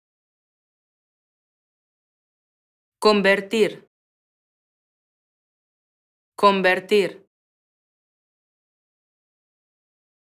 How to pronounce CONVERTIR in European Spanish
This video show you how to pronounce CONVERTIR in European Spanish. Speaker has an accent from Madrid, (central Spain). Collins Dictionary, the home of living language and pioneers of dictionary publishing